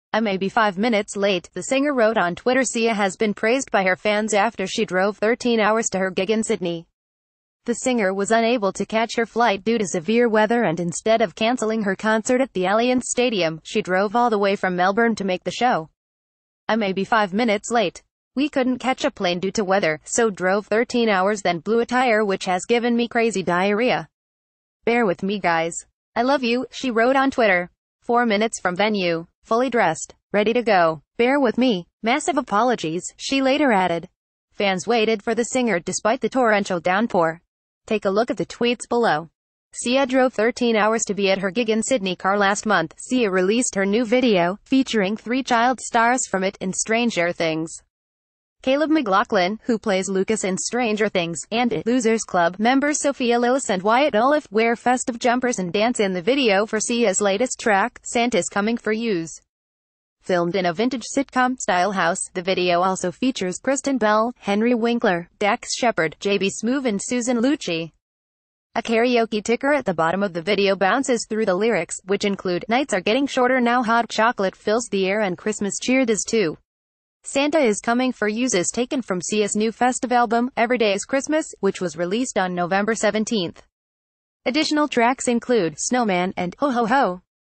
TOP NEWS - SIA drove 9 hours of her performing in sydney after her flight was cancelled
TOP NEWS HEADLINES DAILY - TOP NEWS - SIA drove 9 hours of her performing in sydney after her flight was cancelled

I may be 5 minutes late, the singer wrote on Twitter Sia has been praised by her fans after she drove 13 hours to her gig in Sydney. The singer was unable to catch her flight due to severe weather and instead of cancelling her concert at the Allianz Stadium, she drove all the way from Melbourne to make the show. “I may be five minutes late. We couldn’t catch a plane due to weather, so drove thirteen hours then blew a tire which has given me crazy diarrhea. Bear with me guys. I love you,” she wro...